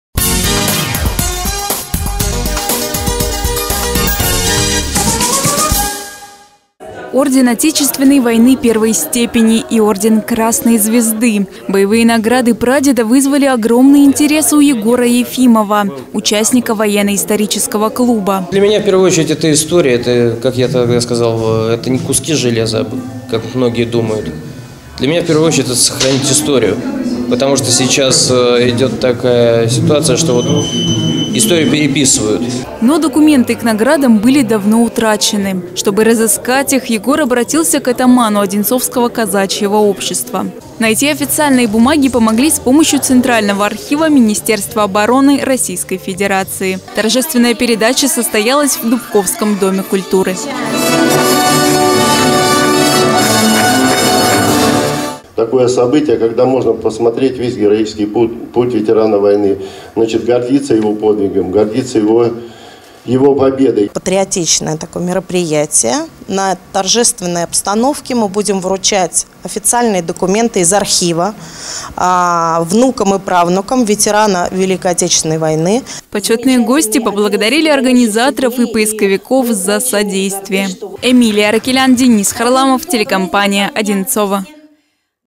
История одного ветерана
В Дубковском Доме культуры прошла торжественная церемония передачи документов к боевым наградам ветерана Великой Отечественной войны Александра Васильевича Карасёва. Ранее они были утрачены.